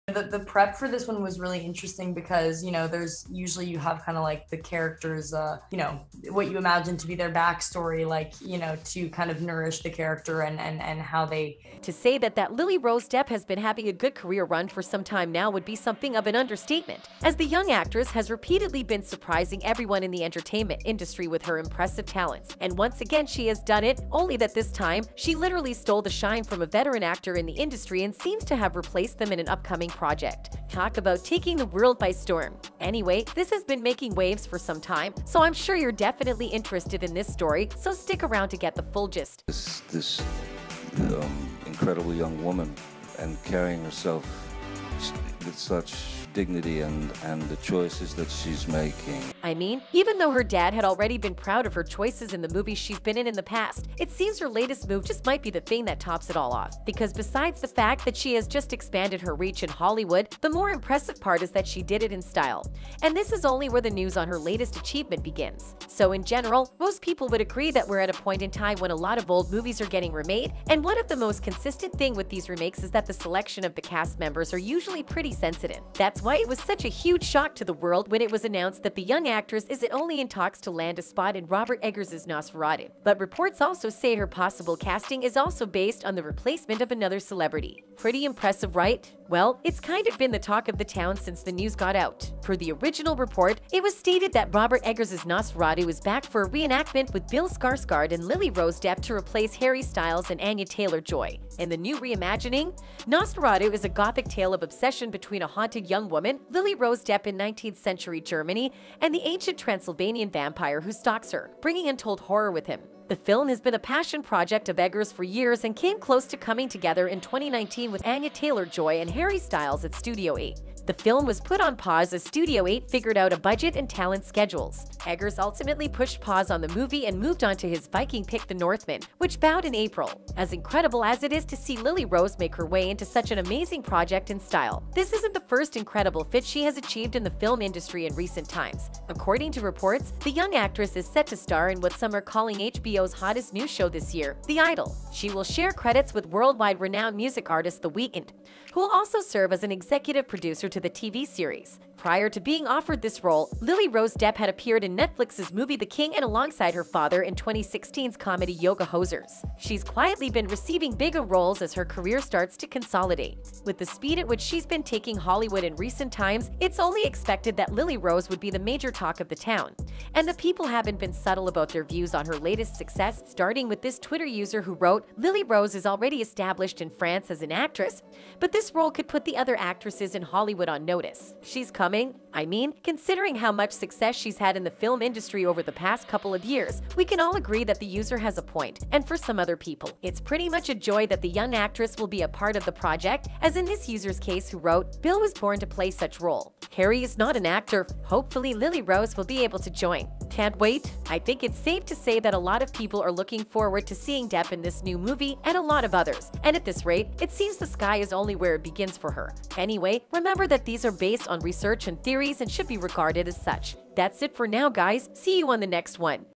Lily-Rose SHOCKS The World As She Replaces HUGE Actor In New Movie!
Lily-Rose Depp SHOCKS The World As She Replaces HUGE Actor In New Movie!
To say that that Lily-Rose Depp has been having a good career run for some time now would be something of an understatement, as the young actress has repeatedly been surprising everyone in the entertainment industry with her impressive talents.